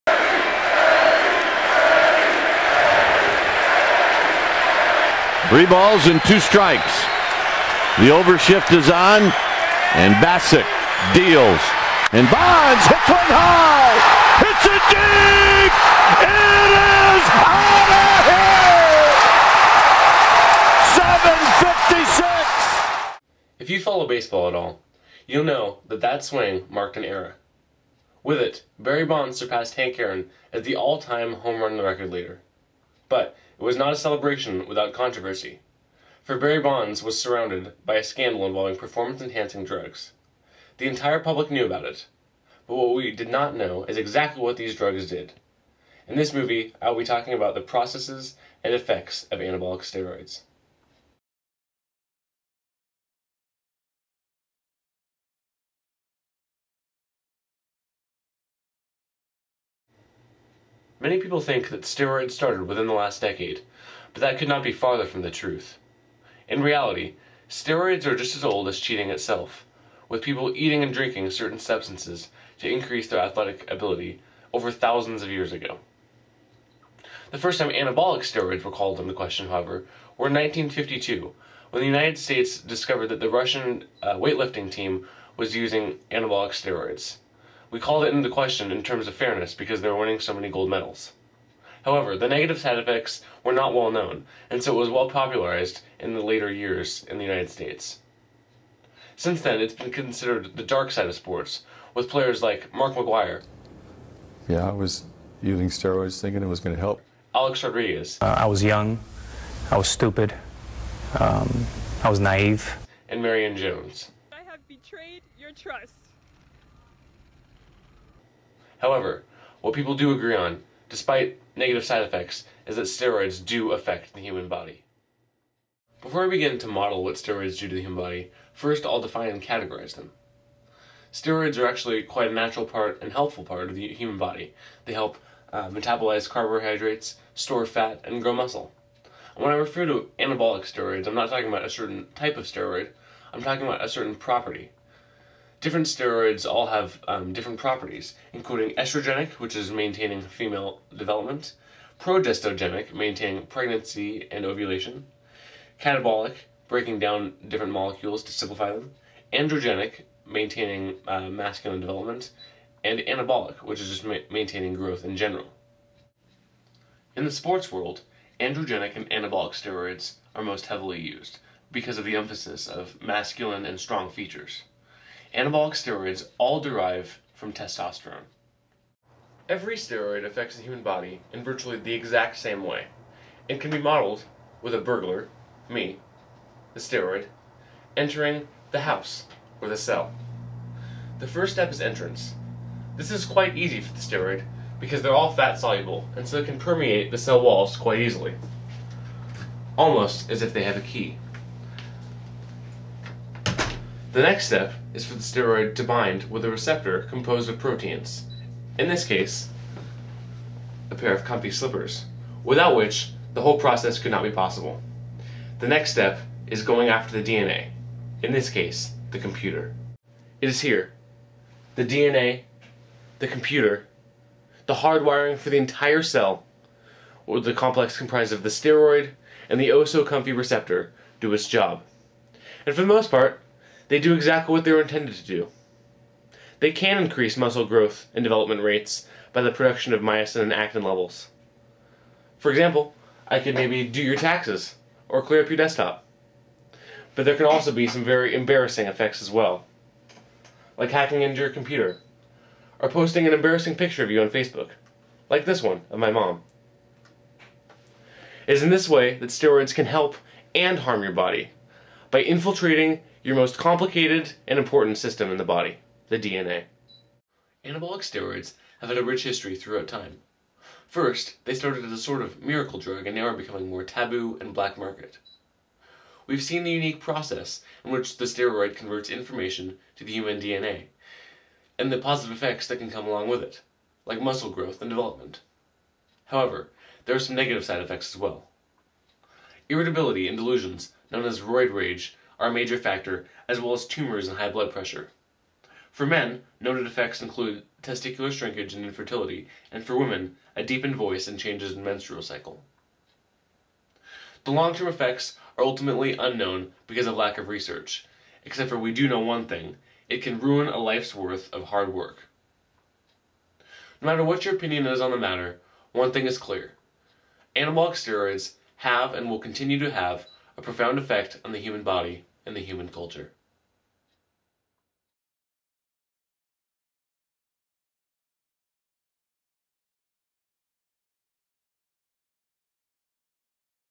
The Truth About "The Juice" (Process and Effects of Anabolic Steroids)
A multimedia project for the 2010 Bio Expo by Skylar Shibayama.  This video is about the history, process, and effects of anabolic steroids.